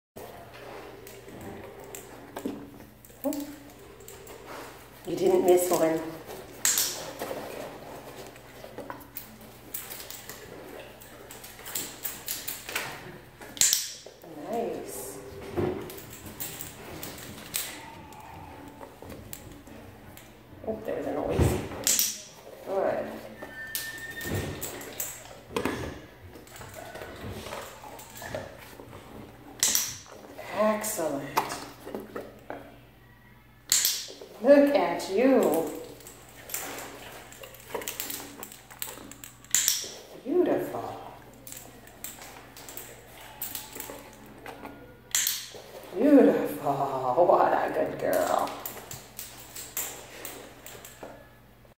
early training for chin rest at vet clinic
Introducing the training of a chin rest for veterinary exams.